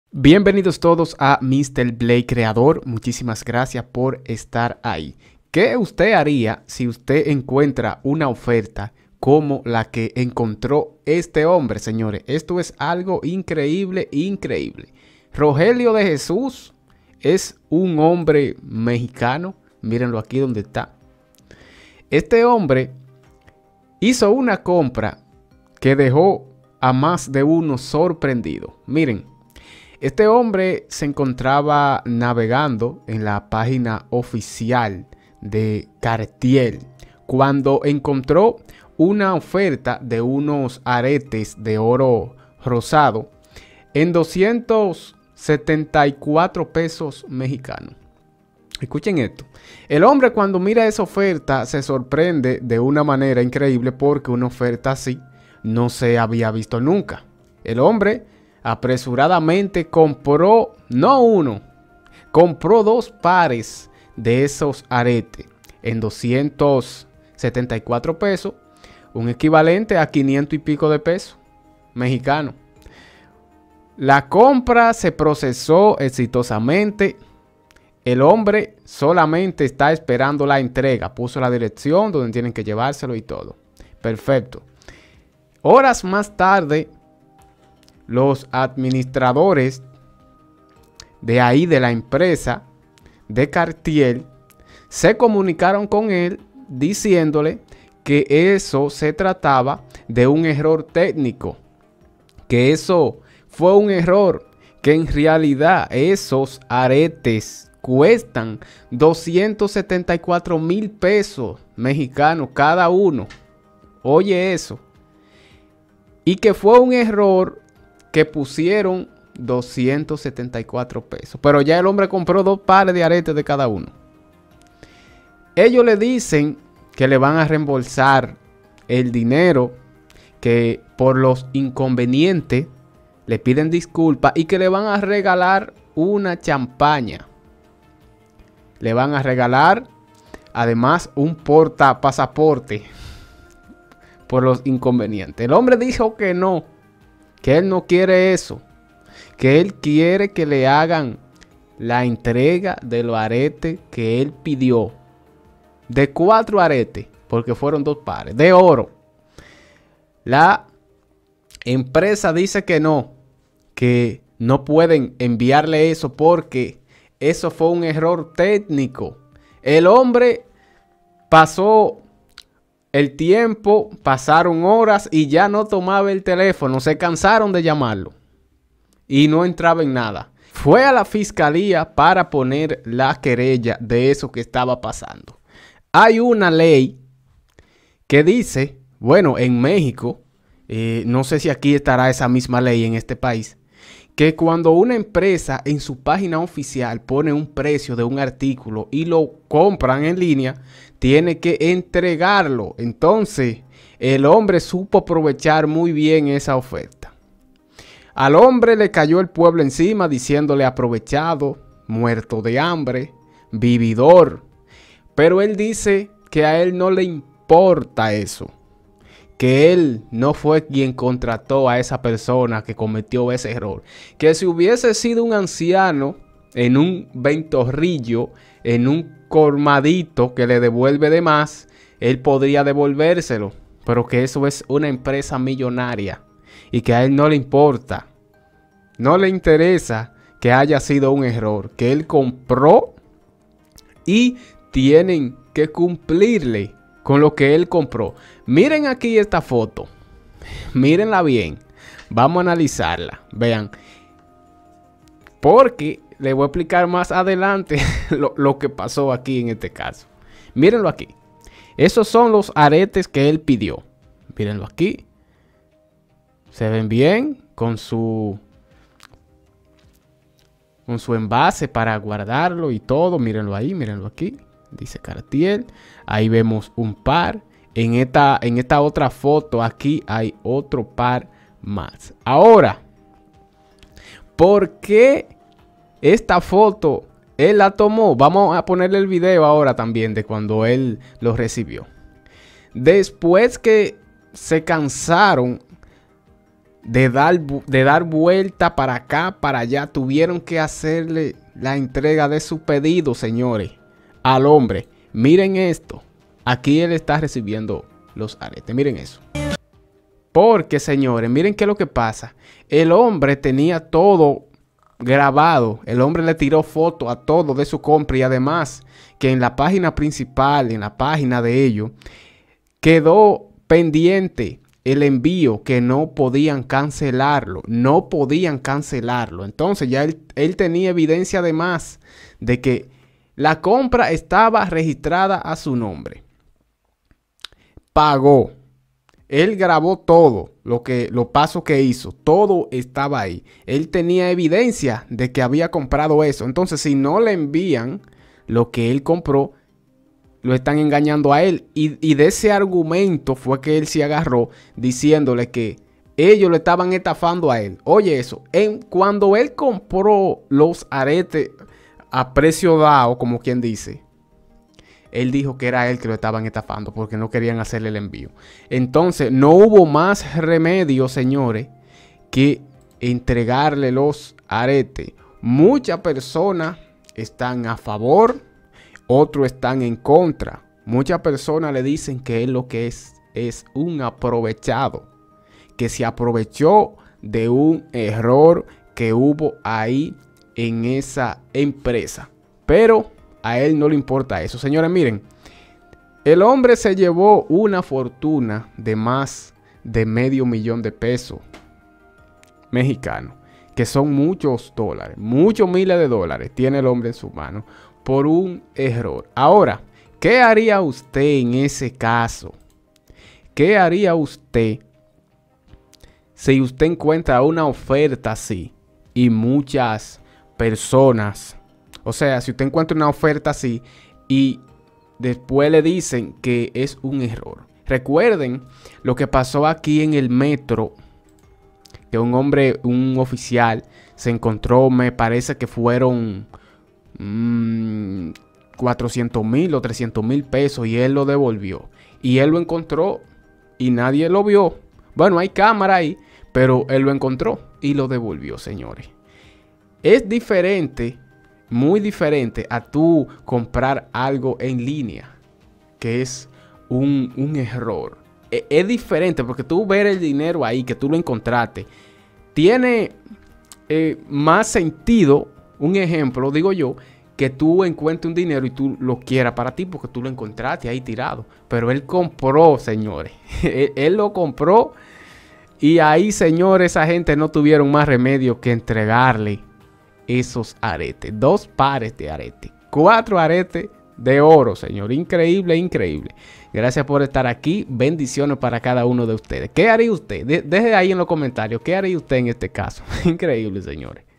Joven compra aretes Cartier en $ 2074 pesos por un error
Gracias a un error, un joven mexicano logró comprar unos lujosos aretes Cartier a solo 274 pesos , los cuales están elaborados en oro rosa de 18 quilates y engastados con diamantes con talla brillante.